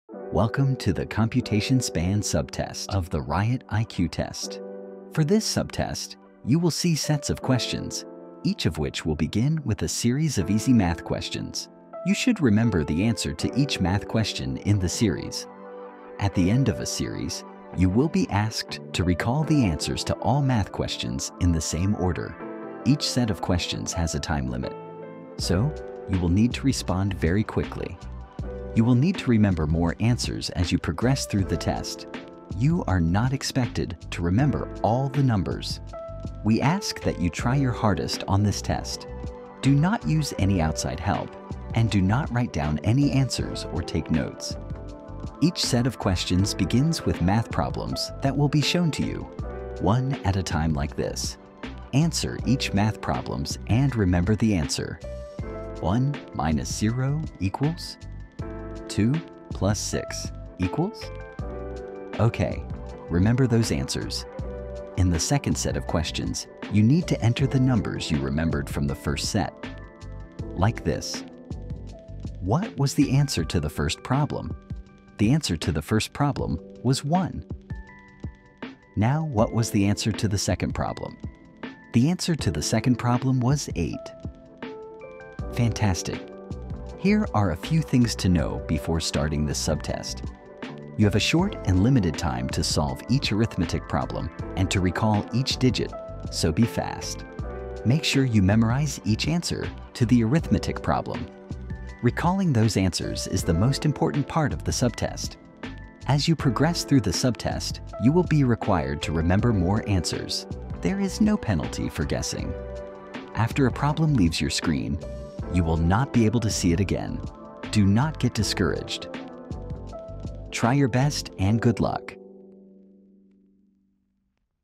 Computation Span Subtest (Working Memory)
Measures working memory capacity and arithmetic skills. Examinees perform mental calculations and are then asked to recall numerical results in the correct order, testing their ability to process and store information simultaneously.